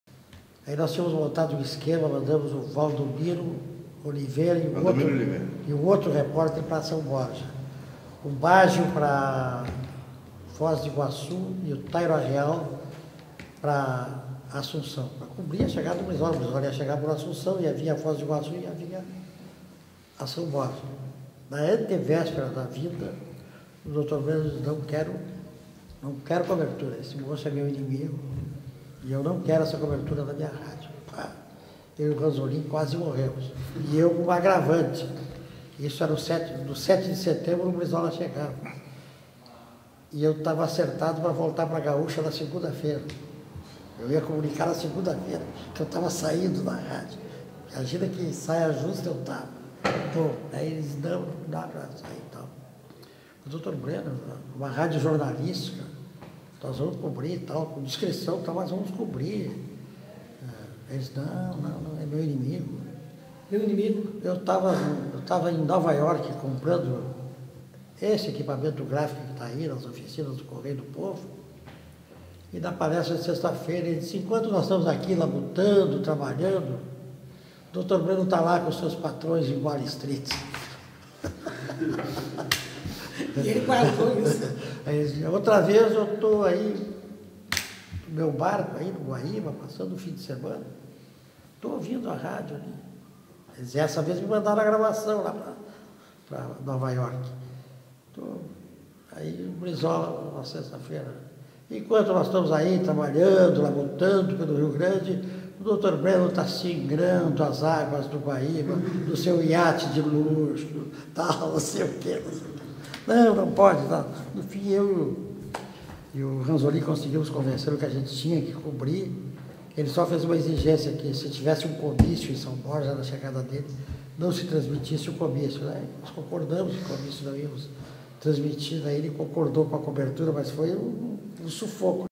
Saia justa na cobertura da volta de Brizola ao Brasil.wmv
ESPECIAL LEGALIDADE: Entrevista com o jornalista Carlos Bastos